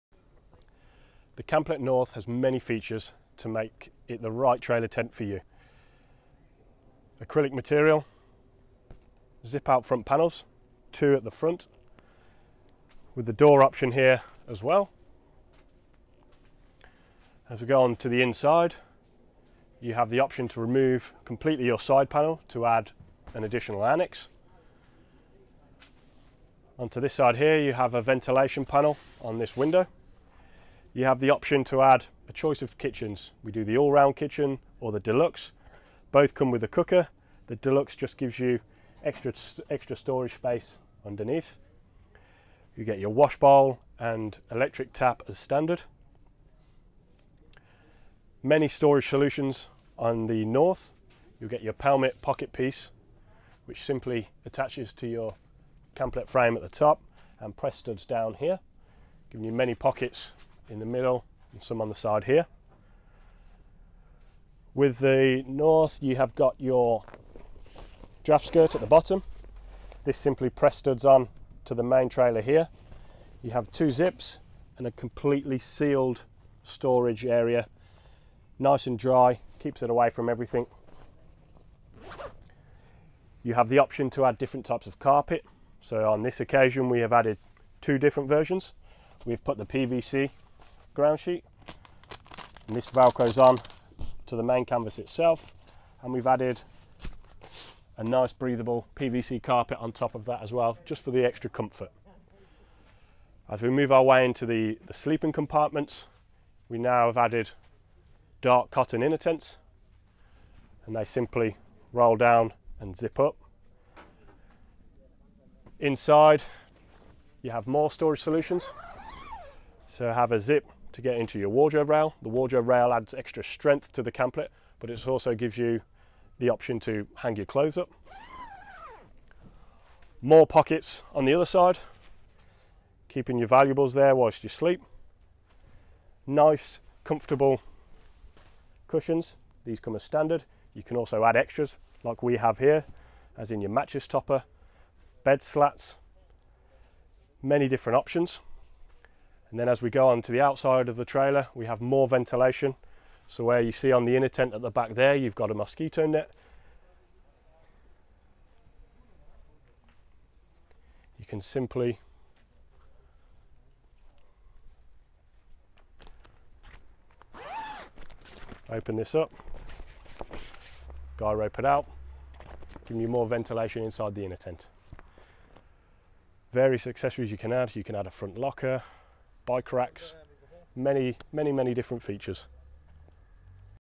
North Overview (English)
Looking for freedom, being able to spontaneously take off in search of new adventures? Camp-let North trailer tent is the perfect choice when you want to enjoy the countryside in first class. Integrated poles mean you can quickly put the Camp-let North up and take it down with a few basic steps.

Camp-let North has room for the whole family. The spacious lounge area and ventilated sleeping cabins with room for 4 give you no less than 18 m2 of holiday accommodation. If you decide to stay a while somewhere, you can quickly extend with a sun canopy and annex.

With its canvas made of the unique Isacryl material, Camp-let North is easy to clean. The material keeps its colour for years, creating the optimum indoor climate in a Camp-let, so that you can enjoy camping – regardless of temperature or weather.